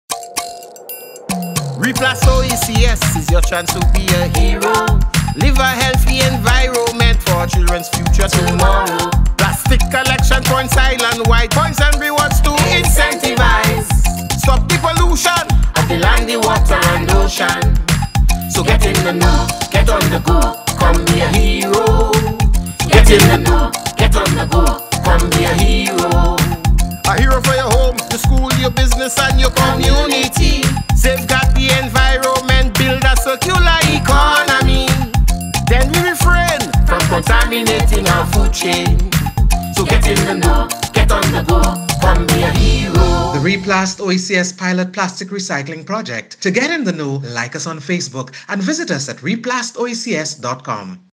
Be a Hero: Reuse and Recycle Plastics (RePlast PSA)
Recycle empty PET and HDPE beverage containers. A public service announcement from the RePlast OECS pilot plastic recycling project.